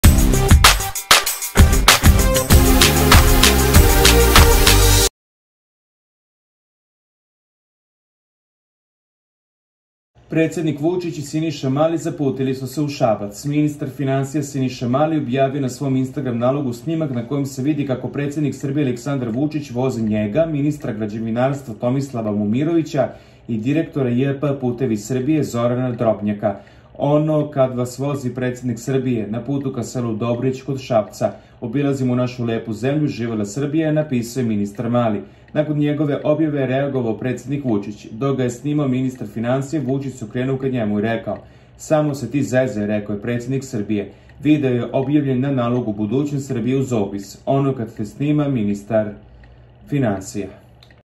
Uzivo! Sinisa Mali OPLEO po Vucicu: TI si nekvalifikovani vozac! Vucic porucio SAMO SE TI ZEZAJ!
Predsednik Vučić i Siniša mali zaputili su se u Šabac.